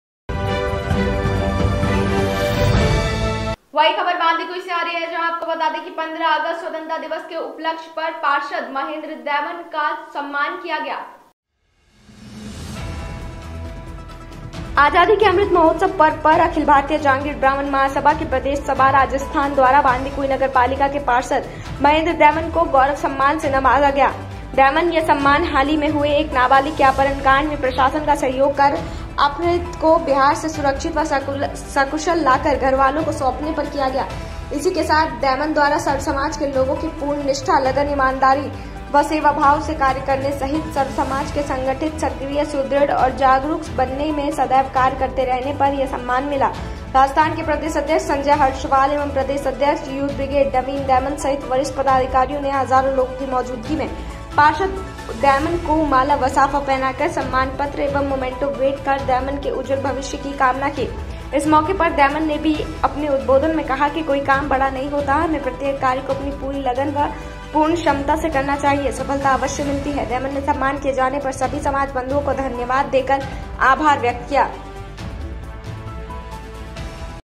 पार्षद महेंद्र दैमन का उत्कृष्ट कार्यों के लिए किया सम्मान
पार्षद महेंद्र दैमन का उत्कृष्ट कार्यों के लिए  किया सम्मान Councilor Mahendra Daiman honored for excellent work